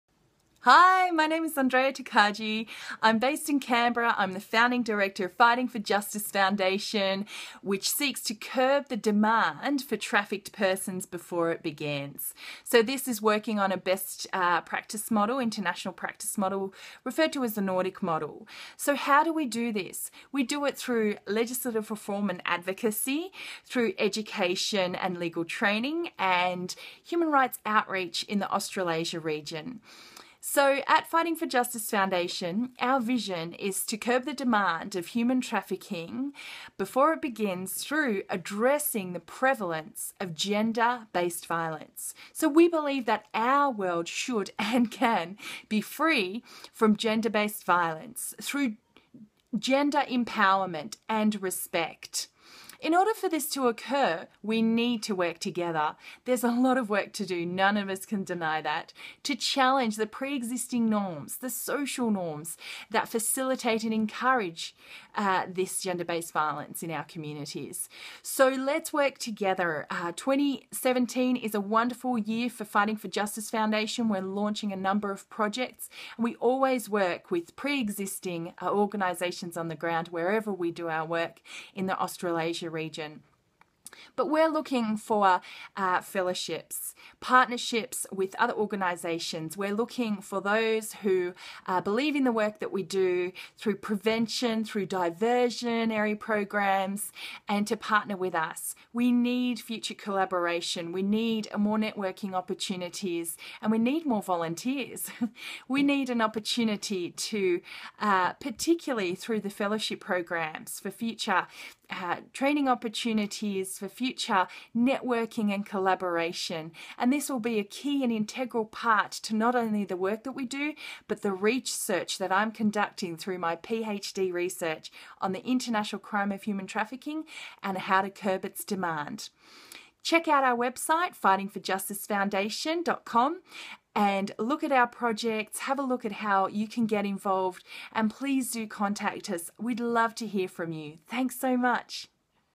Fighting for Justice Foundation - who we are and what we do!
Fighting for Justice Foundation seeks to curb the demand for trafficked persons - before the process begins by addressing gender based violence through advocacy, education and outreach. for more info!